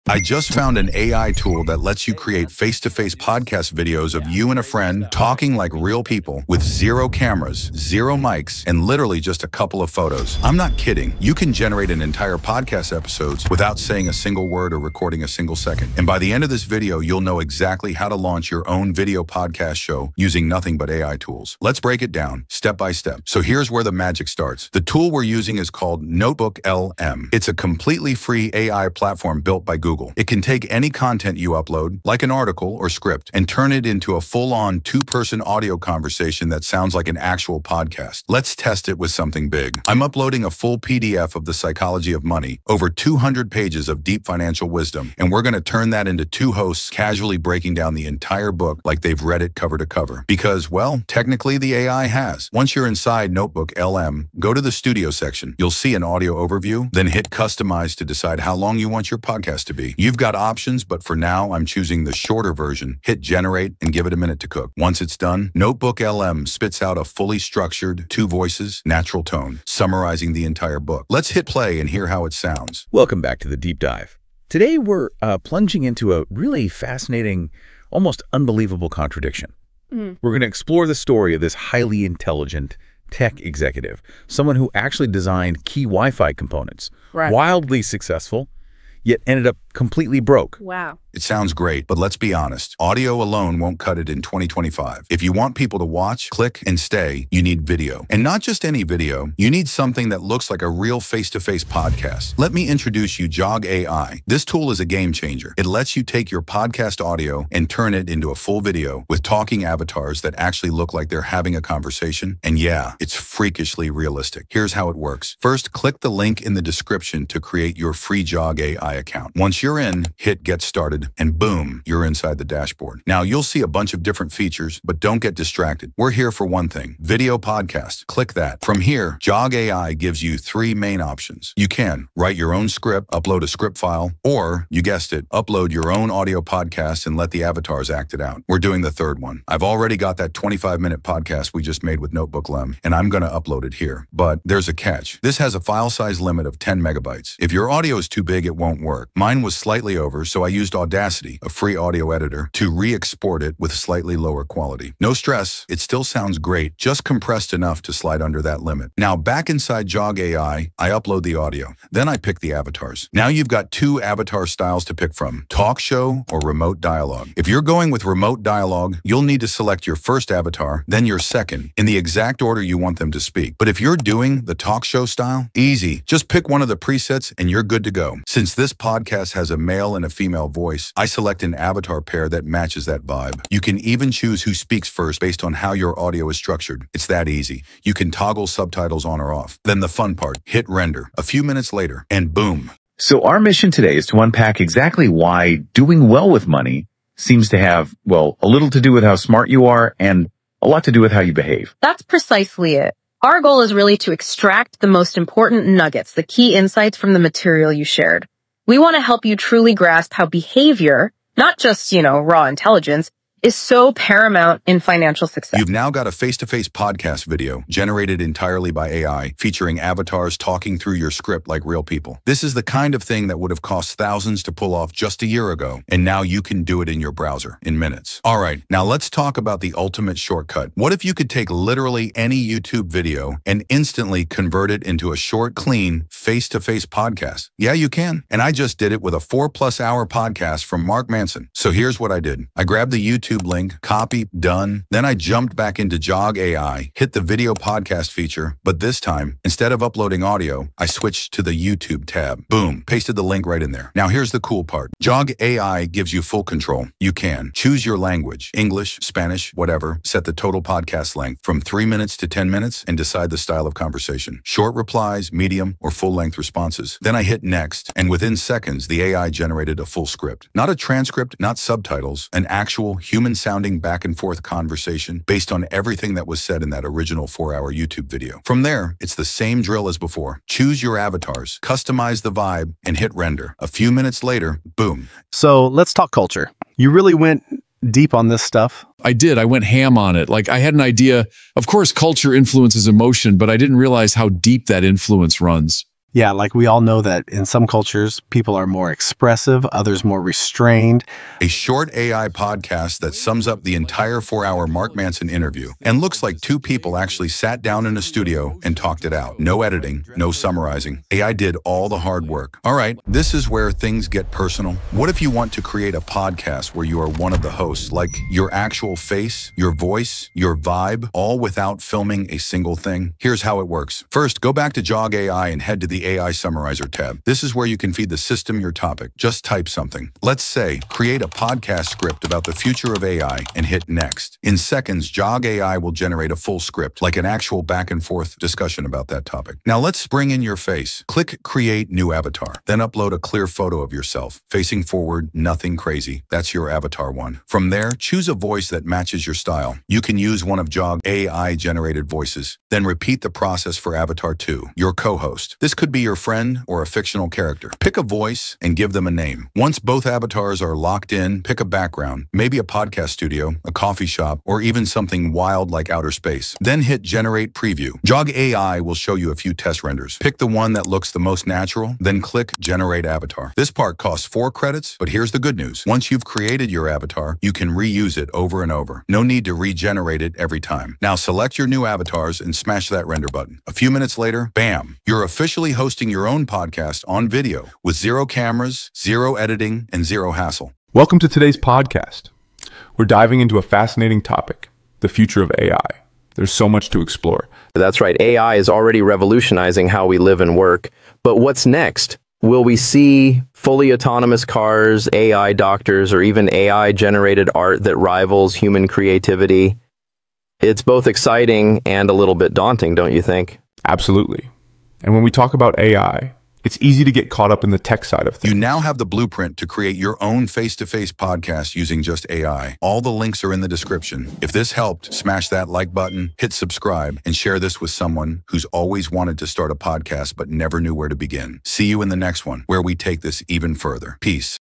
Make REALISTIC Podcasts with AI in JUST 5 Minutes
Make REALISTIC Podcasts with AI in JUST 5 Minutes Using JoggAI
Want to start a face-to-face podcast but don’t have a camera, mic, or editing skills? In this step-by-step tutorial, I’ll show you how to create ultra-realistic podcast videos using just AI tools and a few photos. No filming. No speaking. All automated.

You’ll learn how to:
00:25 Generate a full podcast script using NotebookLM (Google’s free AI tool)
02:28 Audio compression using Audacity
02:38 Convert audio into face-to-face video podcasts with avatars using JoggAI
03:48 Turn long-form content (PDFs or YouTube videos) into natural conversations
05:12 Create podcast hosts using just your own photo and voice selections

You don’t need any technical background—just follow along and by the end, you’ll have a stunning AI-powered podcast that looks and feels real.